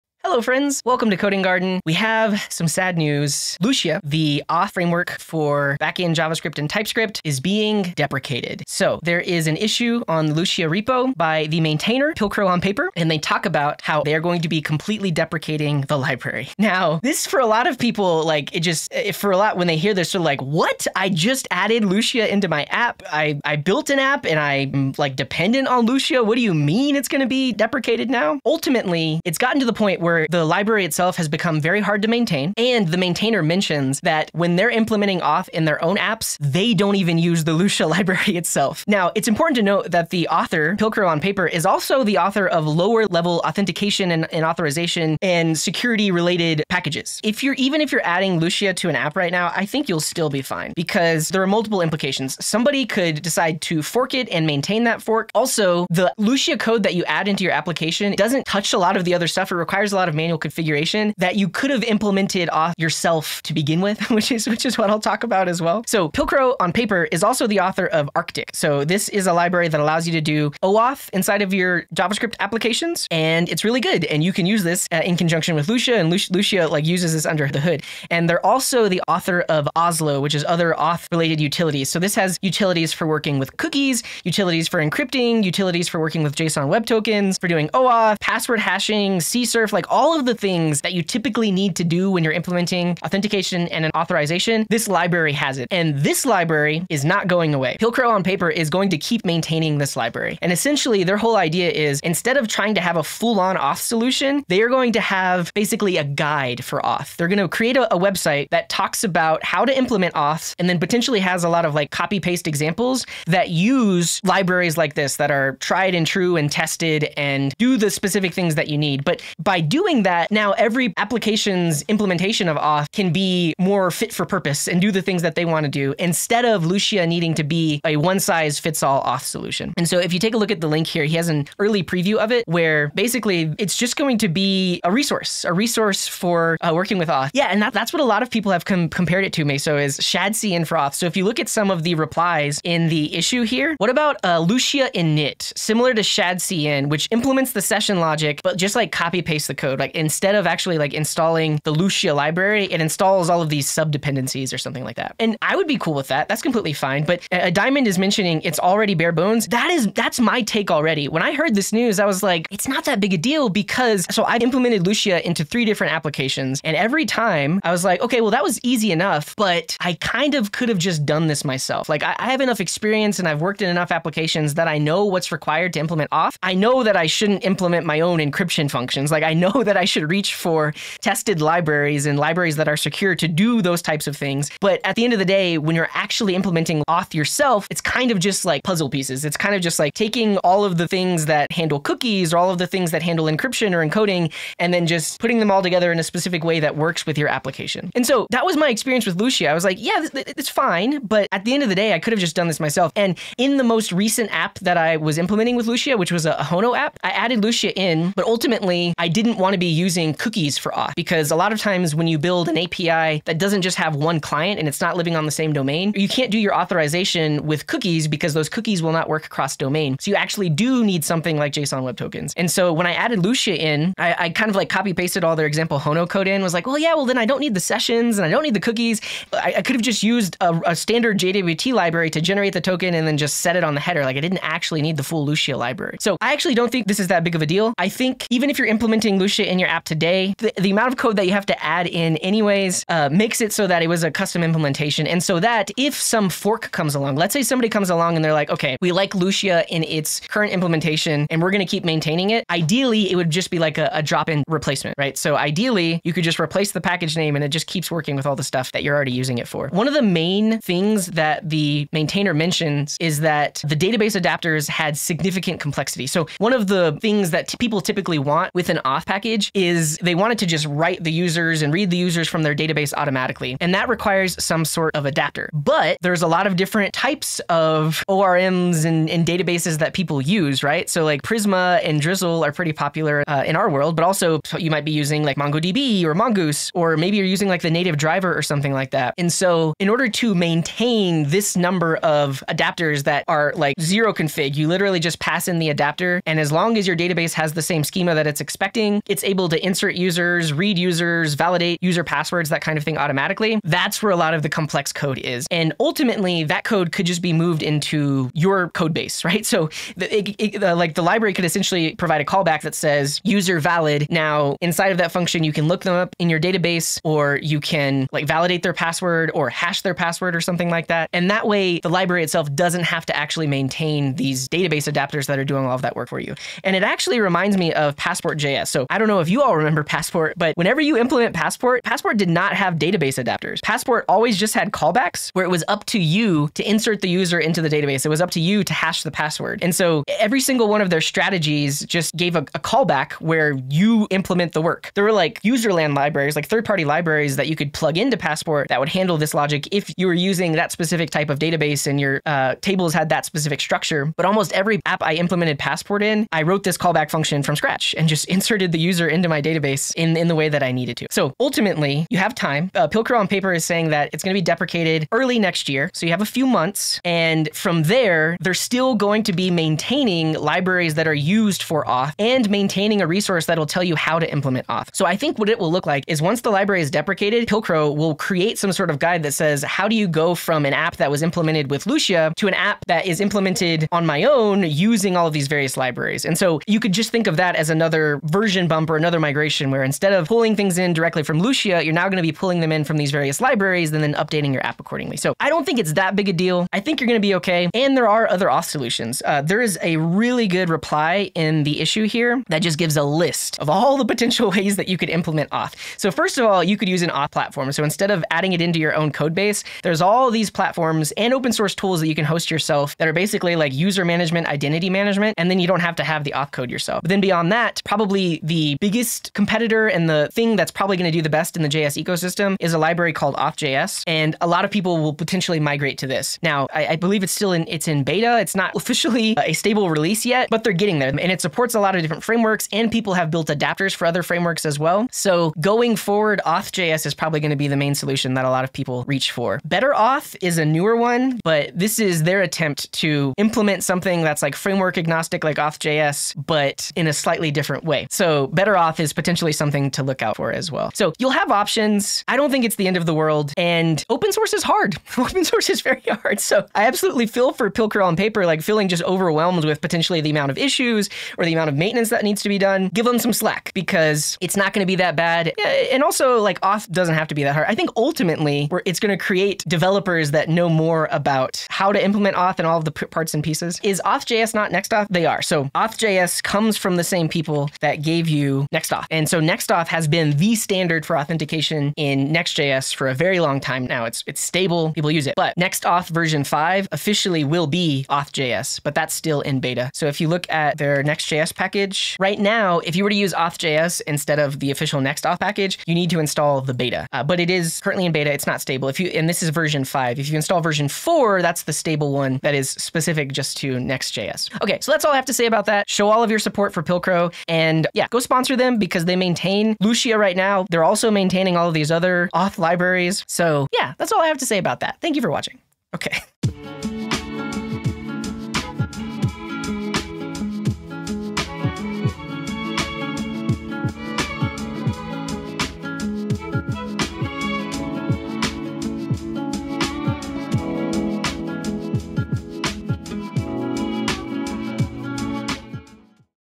Lucia Auth is Being Deprecated
In this video CJ talks about what is happening and what comes next for Lucia Auth.